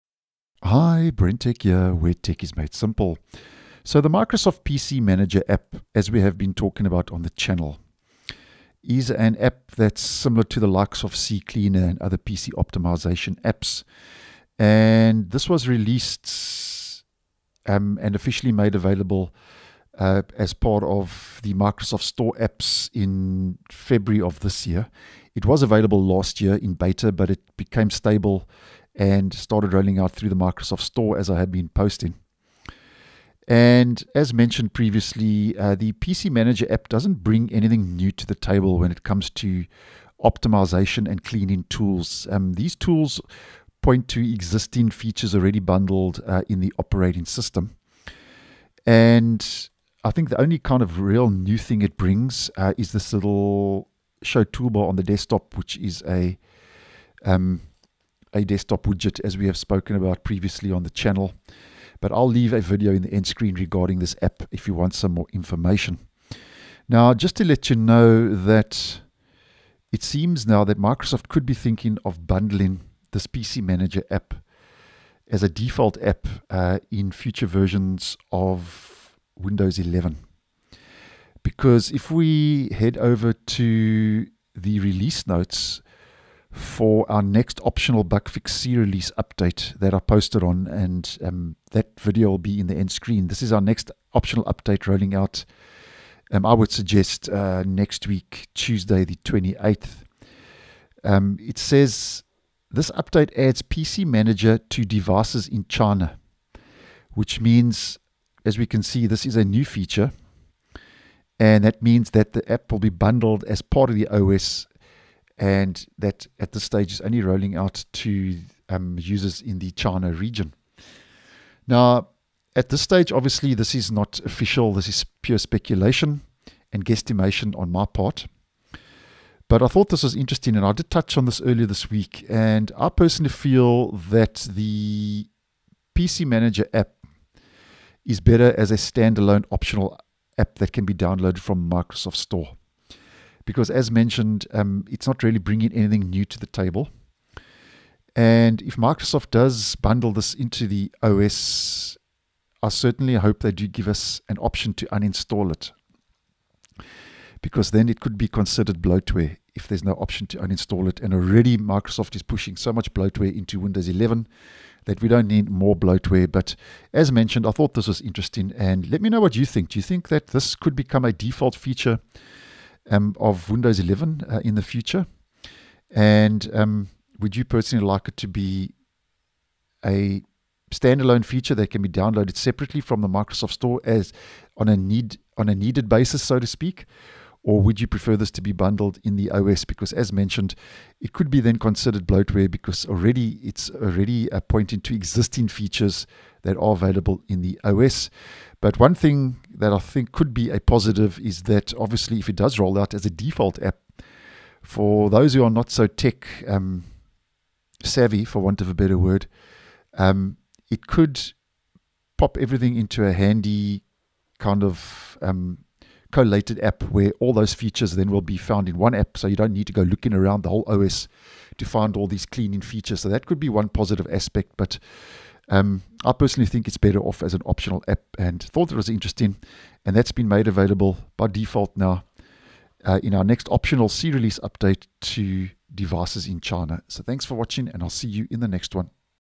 Microsoft PC Manager could become a Default App on Windows 11 - I hope it doesn't
Windows 11 Builds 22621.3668 and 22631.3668 (KB5037853) add PC Manager to devices in China, indicating that Microsoft may be considering bundling it into the OS eventually.